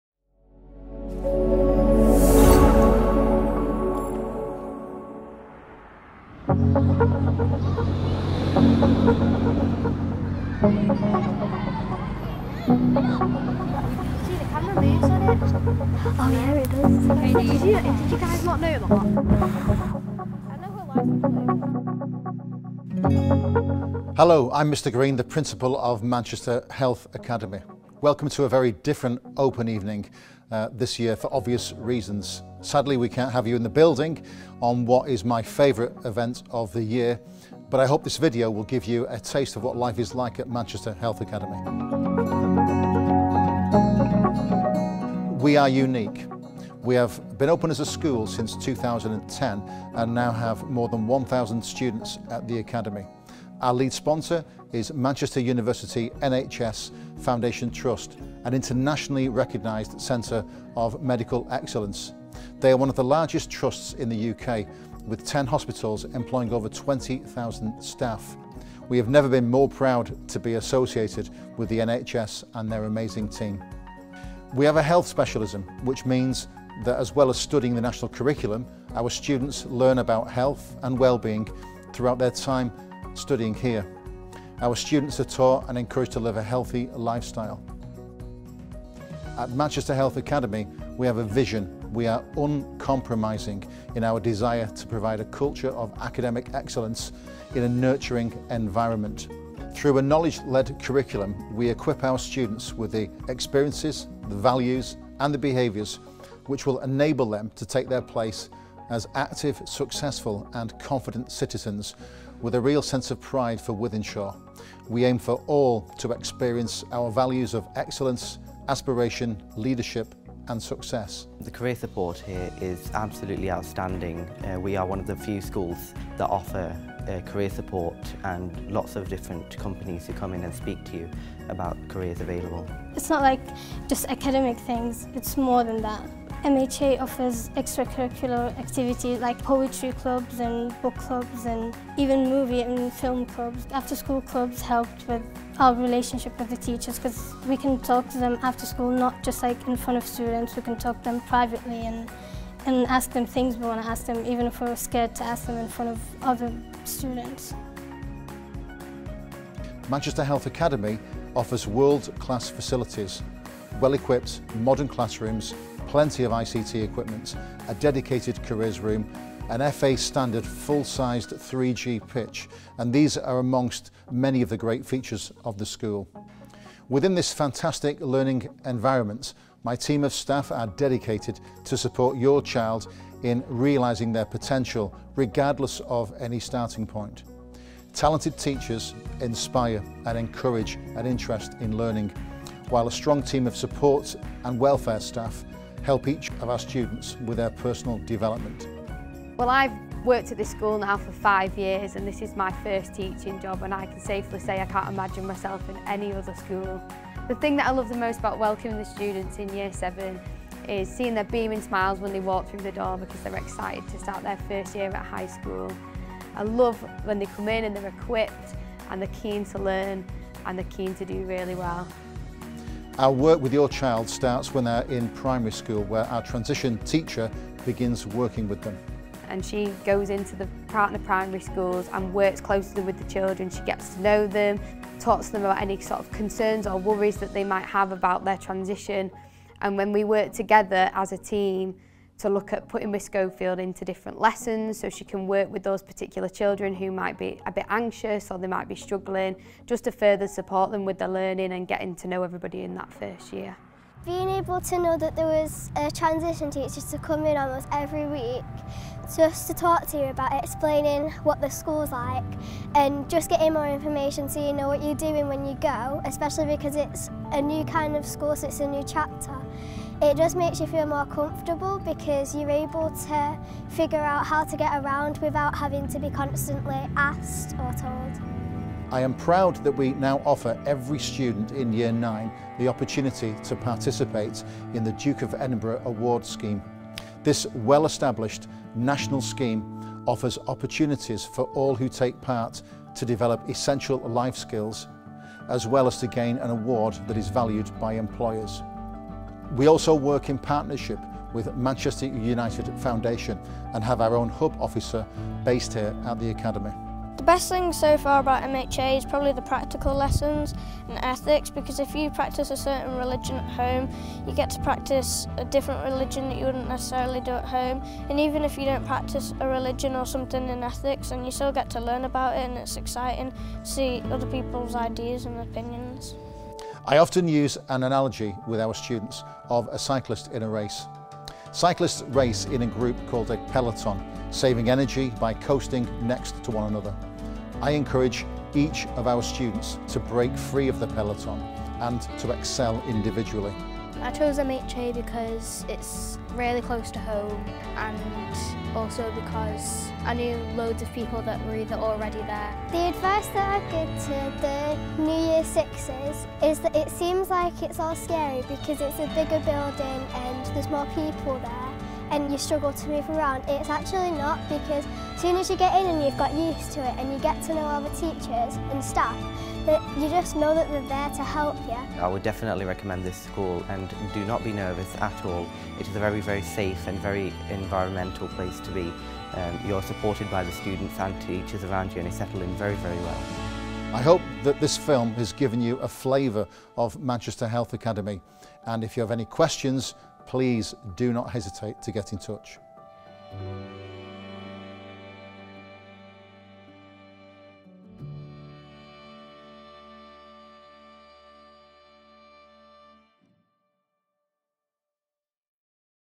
Manchester Health Academy Virtual Open Evening 2020
Make Manchester Health Academy your first choice! Get a feel for our school, what we are about and how we will help nurture and support your child through high school. Meet the Principal and our Director of Standards for Year 7, and hear what some of some of our students think.